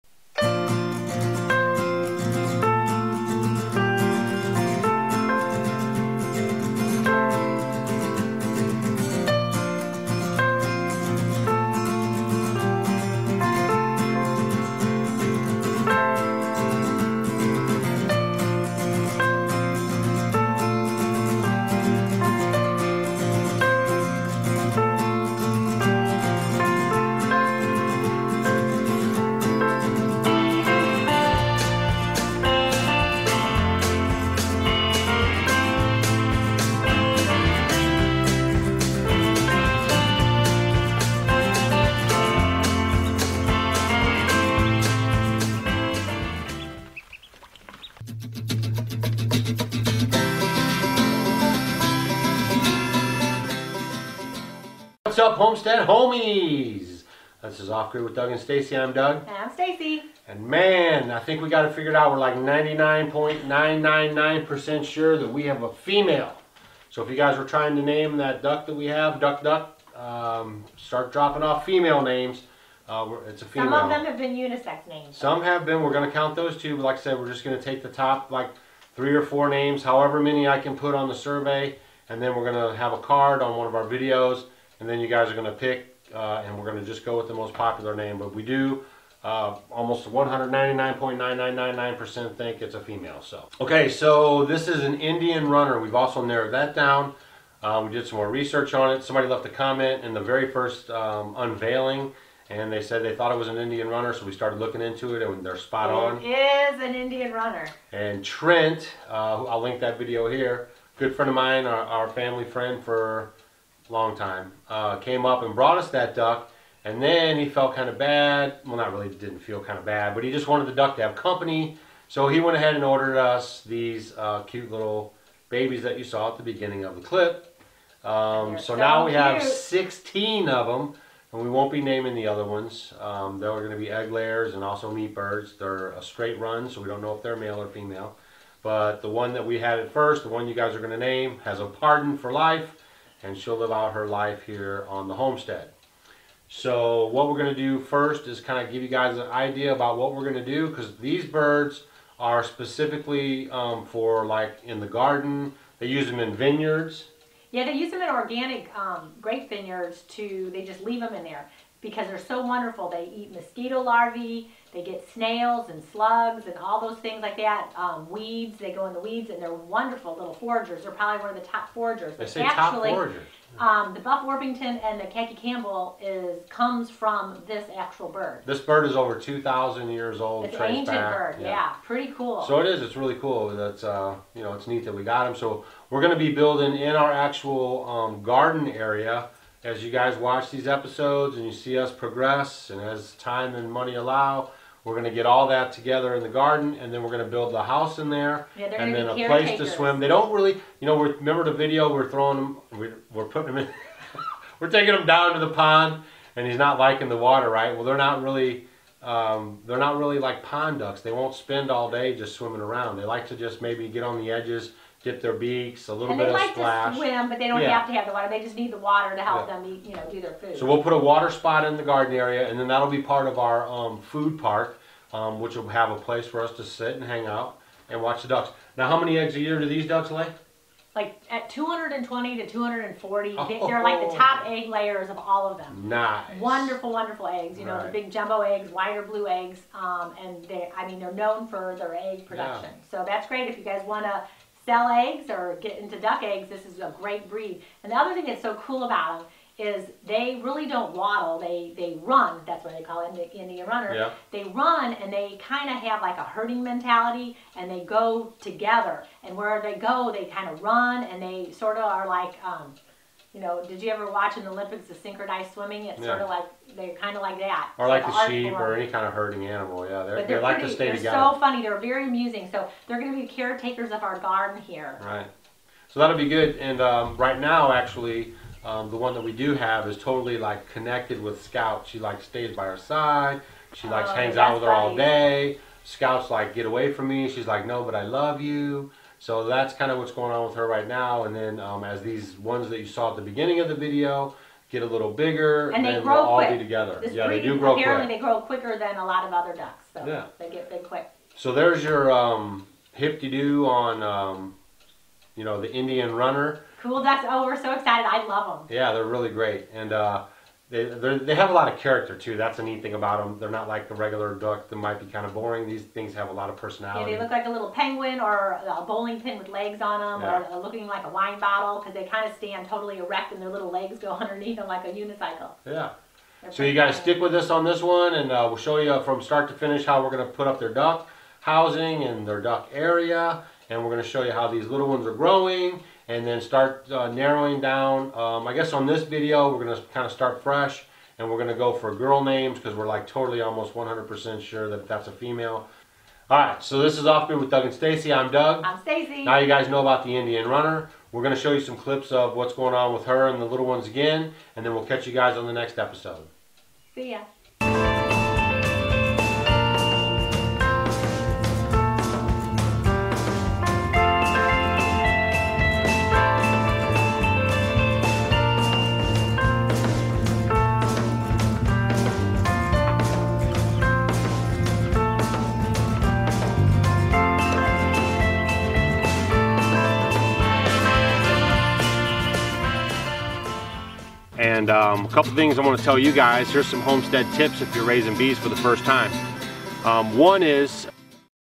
INDIAN RUNNER DUCKS ON THE HOMESTEAD
OFF GRID with DOUG and STACY: Hey Guys, Stacy and Doug have figured out that the duck is an Indian Runner duck.

These ducts lay 200-250 eggs a year and are know for keeping the garden free of unwanted bugs. They use these ducks at vineyards all around the world

They are traced back thousands of years and with out them you would not have a khaki Campbell duck or a few others

Send Mail to:
Bowling Green, MO 63334